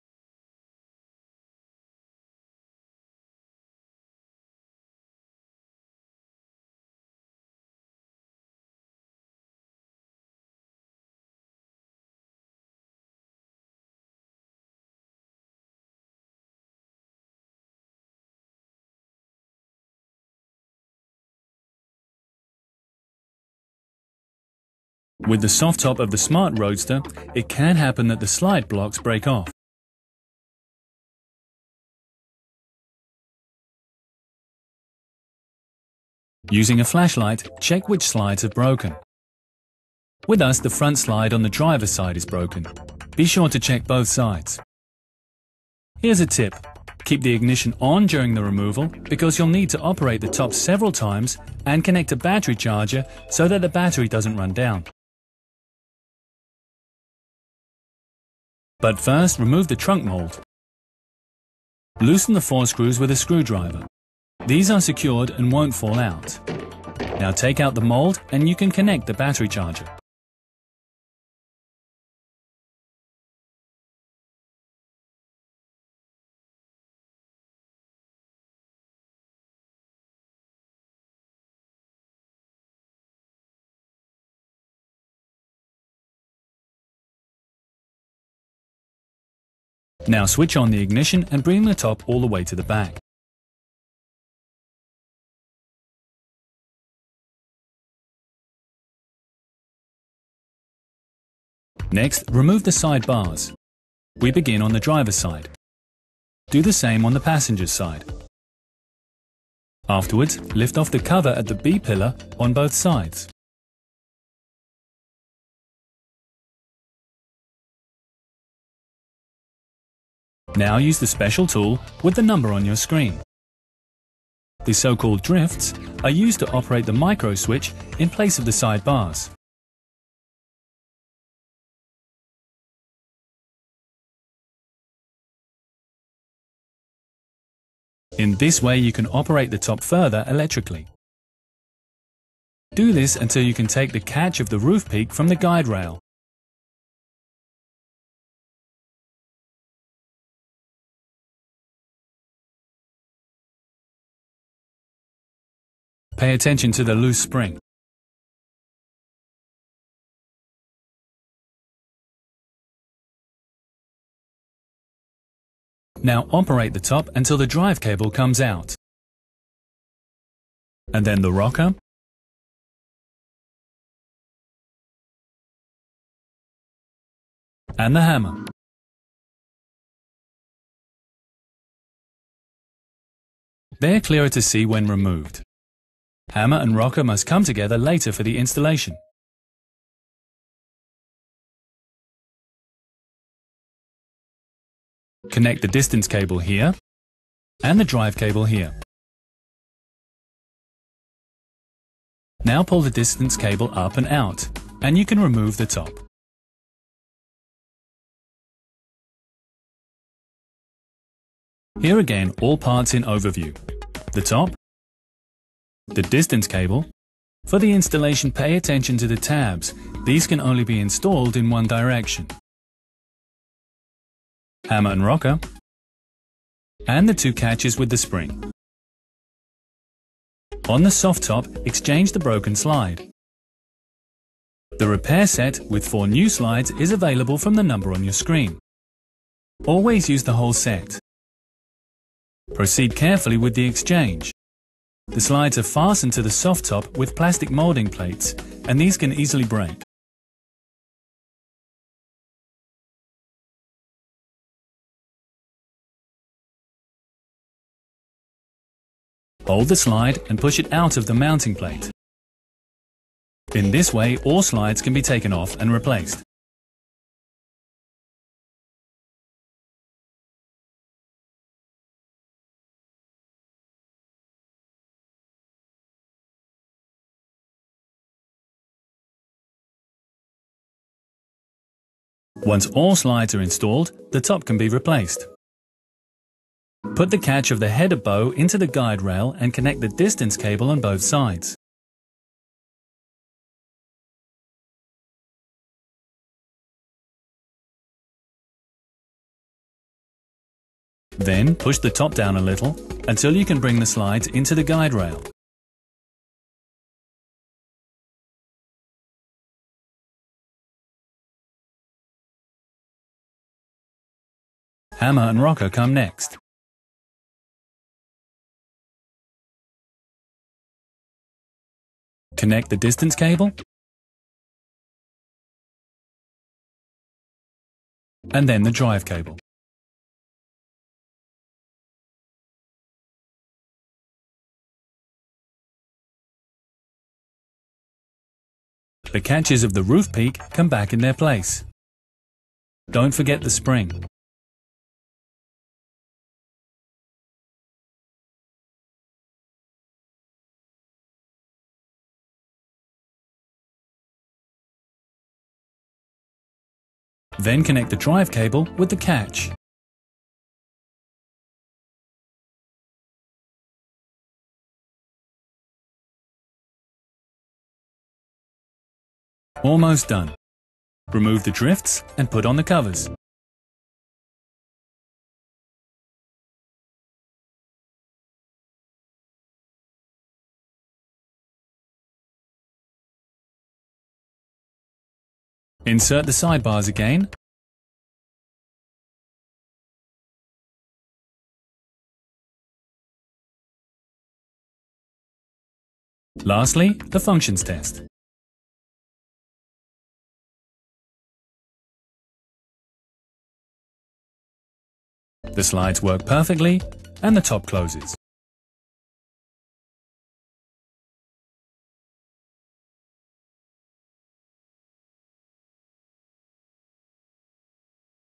Smart Roadster Roof Repair AI Upscale
I took the original 2003 video that smart released and I ran it through a piece of software called Topaz Video Enhance AI. It uses AI to try to recreate the missing data which allows you to upscale the video.

The video went from 320x240 to 1080x1980.

Some of it looks unusual but in general, it's a lot easier to understand compared to the original. The old video had terrible interlacing effects when the picture panned. There's nothing I could do about it.

I had to mute everything except the talking since it was copyrighted.

for my 2019 remake featuring a fantastic moustache.